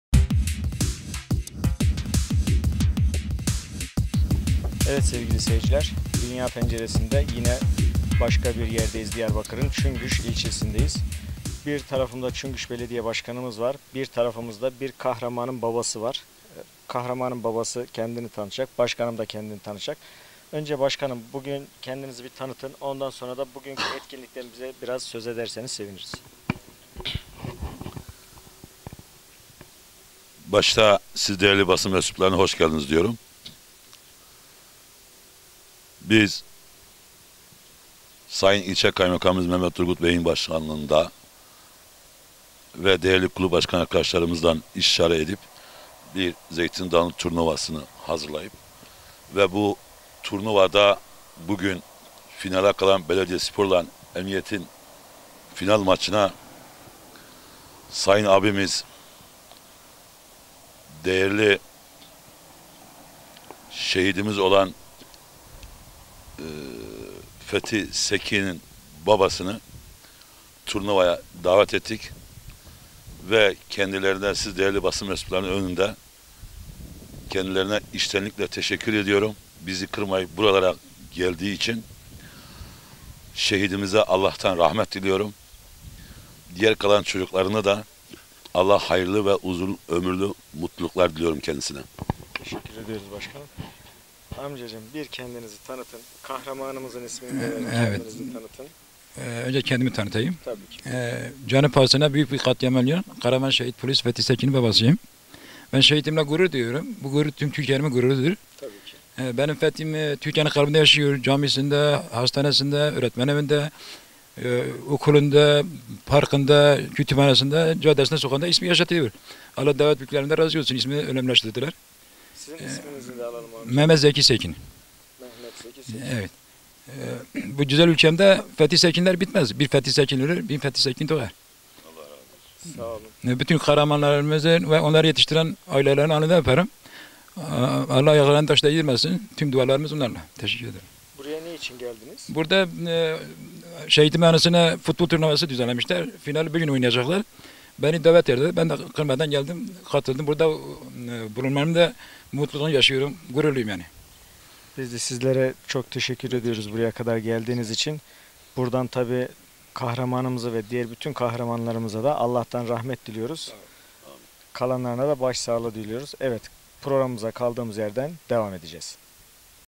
MURAT YILMAZ KURT - KAHRAMAN ŞEHİT POLİS FETHİ SEKİN'İN BABASIYLA SOHBET
Siteler TV Production
Siteler TV Koordinatörü Murat Yılmaz KURT Kahraman Şehit Polis Fethi Sekin'in Babası Mehmet Zeki Sekin ve Diyarbakır Çüngüş Belediye başkanı Zübeyir Arslanca ile kısa sohbeti.